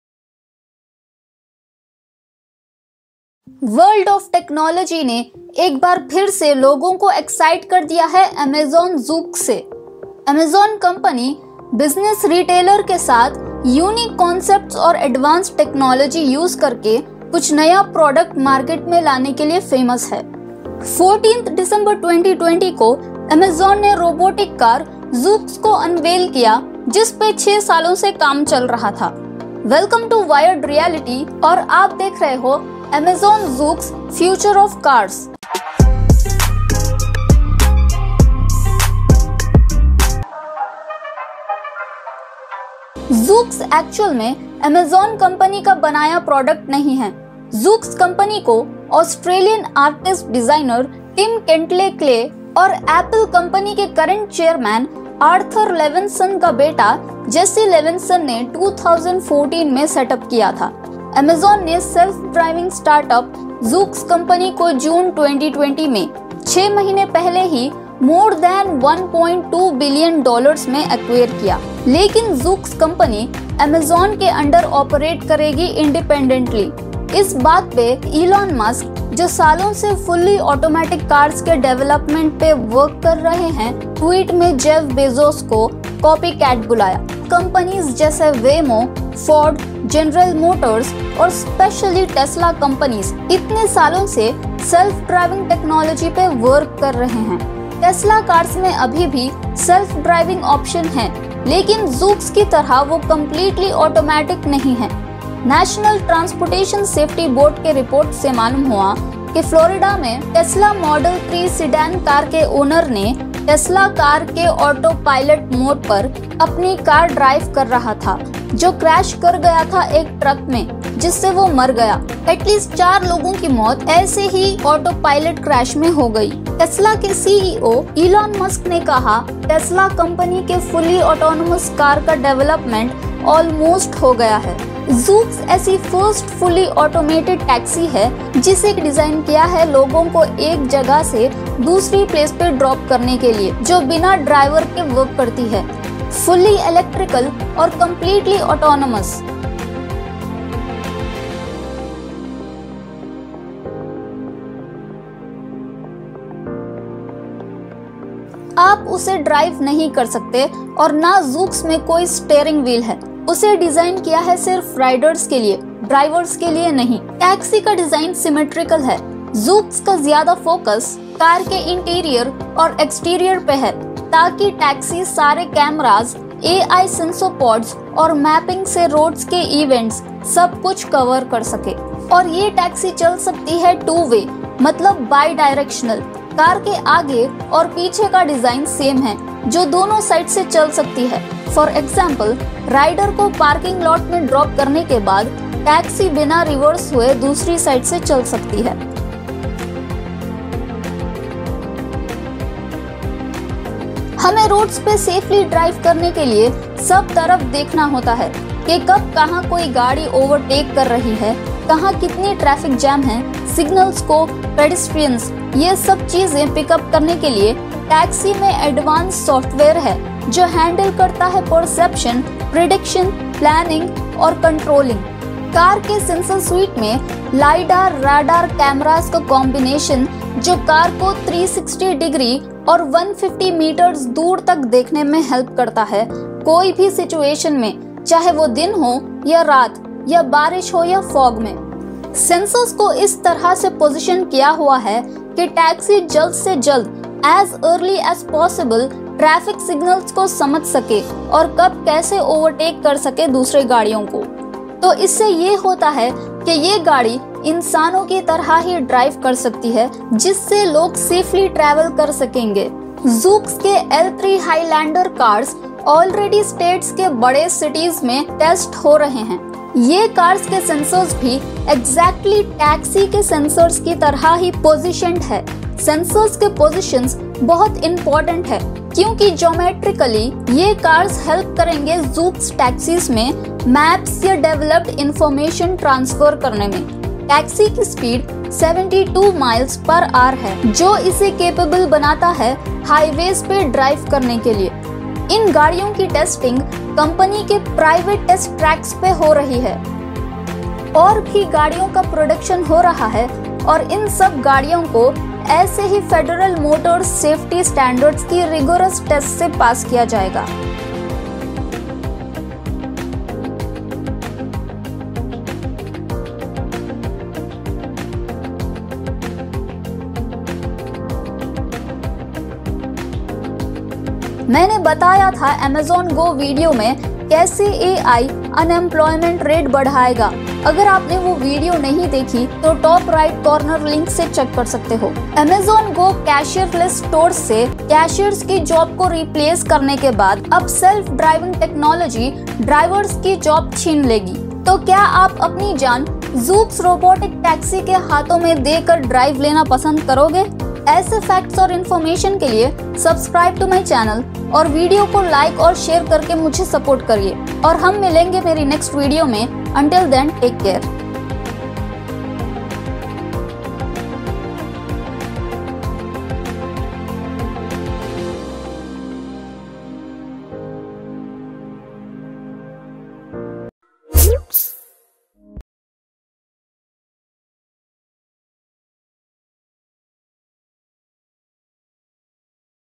FIRST ROBO TAXI | ZOOX | Explained | Future of Cars | Wired Reality
ZOOX have recently unveiled the first fully automating taxi, which would be the future of transportation. this Robo taxi could take over human drivers. it's all electric and fully autonomous built for riders, not drivers.

Voice Over and Edited by - Ms Sameen